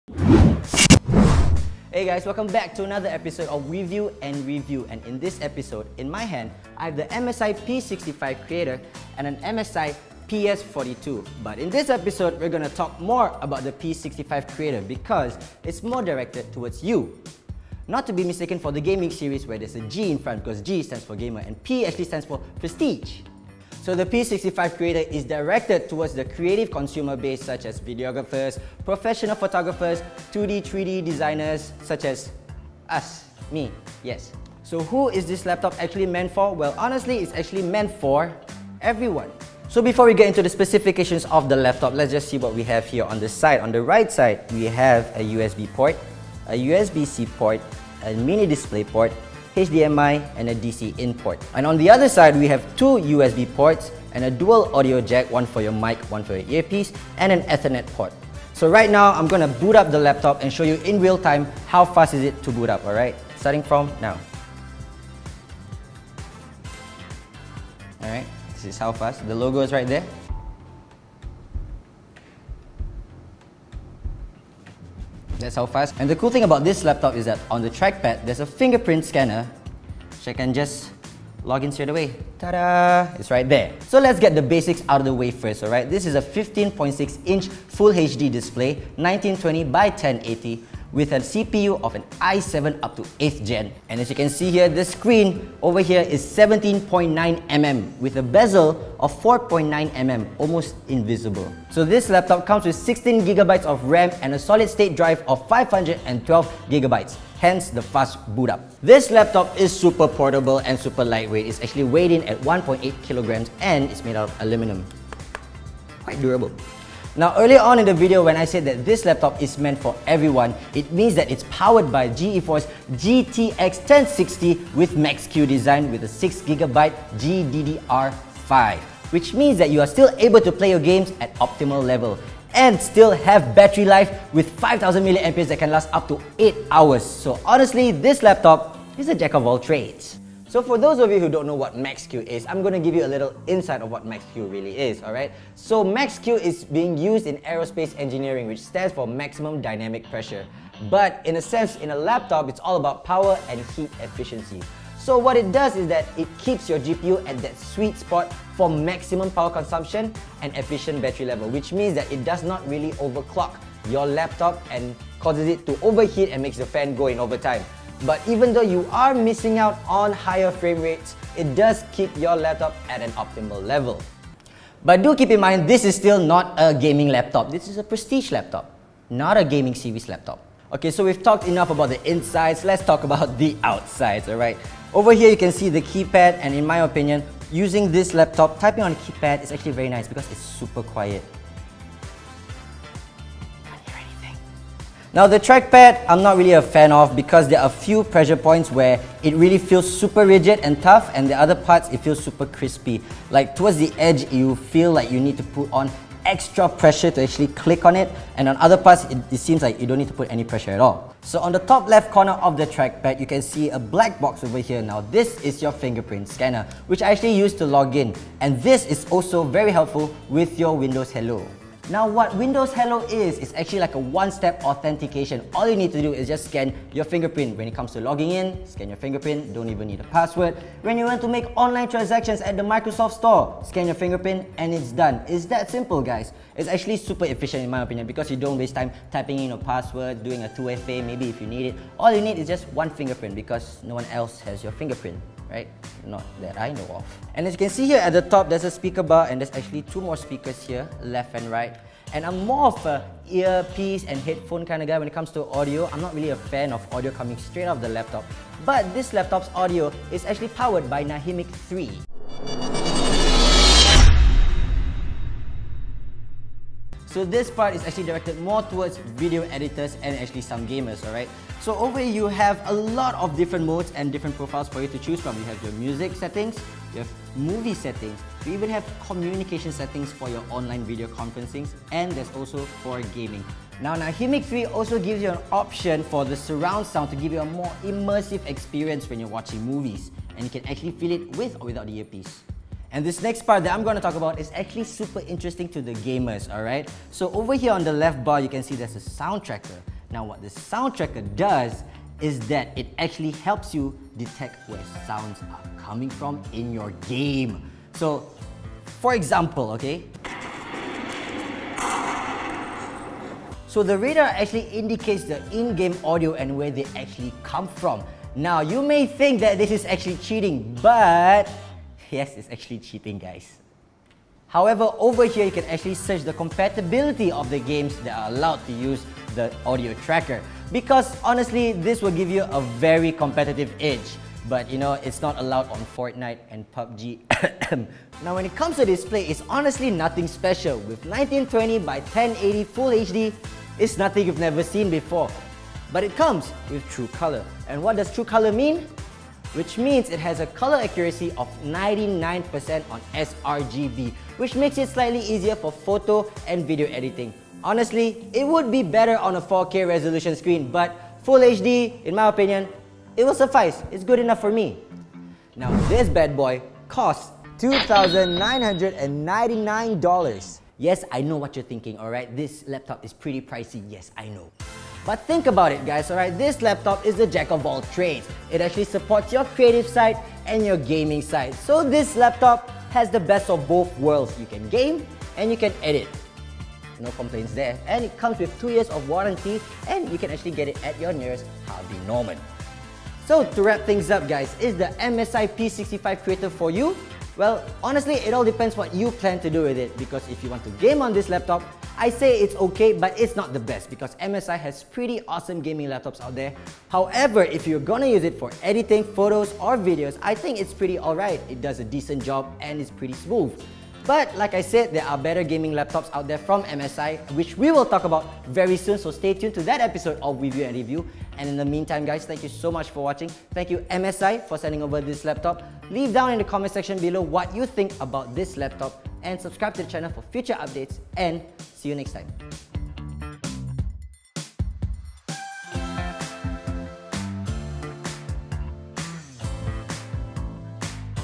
This MSI Laptop is Made For Everyone - P65 Creator
P65 Creator : Built For The Creative Minds | MSI

The thin bezel display granted an even more compact design: ultra-slim 17.9mm and light 1.88kg, with up to latest 8th Gen. Intel® CoreTM i7 processor, NVIDIA® GeForce® GTX 1070. MSI P65 Creator is powerful and sleek in one. With True Color technology providing close to 100% sRGB panel and exclusive cooling system, MSI P65 outperforms the competition. 8+ hours battery life and the fingerprint sensor are helpful to creators.